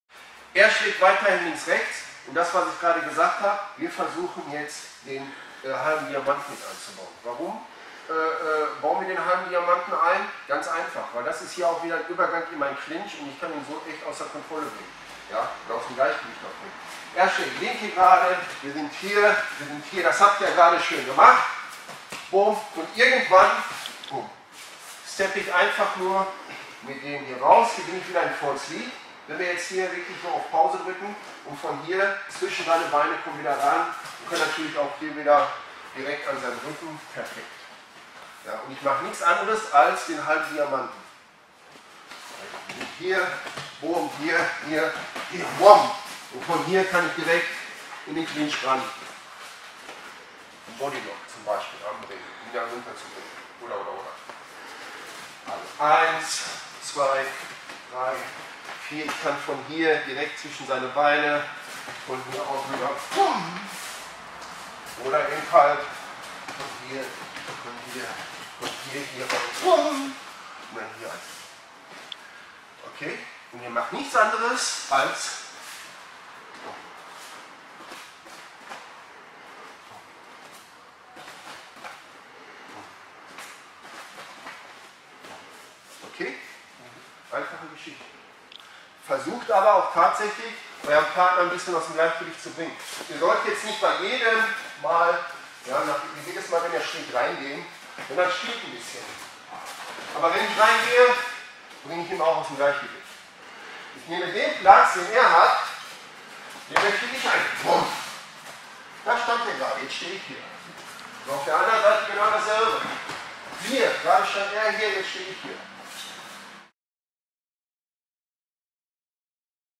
Filipino Boxing - Diamond and Off Balance
WARRIORS PANANTUKAN...

Ob Panantukan (Philippinen), Muay Thai (Thailand), Lethwei (Myanmar), Kun Khmer / Pradal Serey (Kambodscha), Silat (Indonesien / Brunei), die südostasiatischen Stile ähneln sich allein schon durch die geographische Lage, Handelsbeziehung, Krieg etc. und haben sich so auch untereinander beeinflusst.

BATTLE TESTED - COMBAT READY!